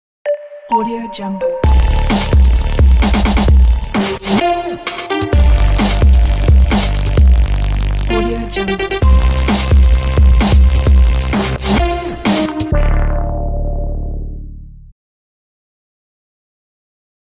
Fast Opener | After Effects Project Files - Videohive template
No plugin required Full HD 1920×1080 Fast Render Easy to modify and customize background – simple for use, color controller added Easy customization \ Change logo, pictures, text and render Created in AE CS5 (CS5 and above) Pdf tutorial included You can buy music here HedanFratze